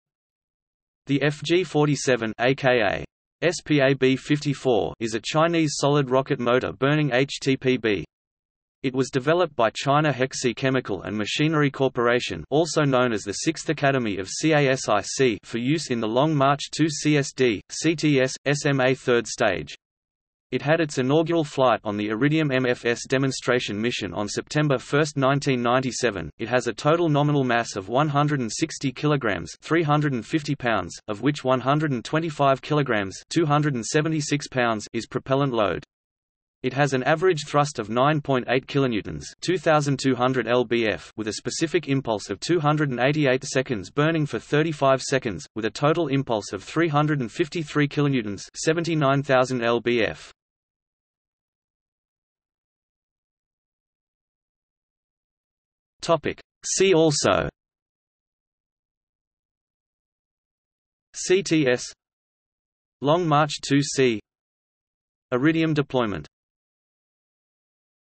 FG-47 | Wikipedia audio article
00:00:52 See also

- Socrates

SUMMARY
The FG-47 (a.k.a. SpaB-54) is a Chinese solid rocket motor burning HTPB. It was developed by China Hexi Chemical and Machinery Corporation (also known as the 6th Academy of CASIC) for use in the Long March 2C SD/CTS/SMA third stage. It had its inaugural flight on the Iridium-MFS demonstration mission on September 1, 1997.It has a total nominal mass of 160 kg (350 lb), of which 125 kg (276 lb) is propellant load. It has an average thrust of 9.8 kN (2,200 lbf) with a specific impulse of 288 seconds burning for 35 seconds, with a total impulse of 353 kN (79,000 lbf).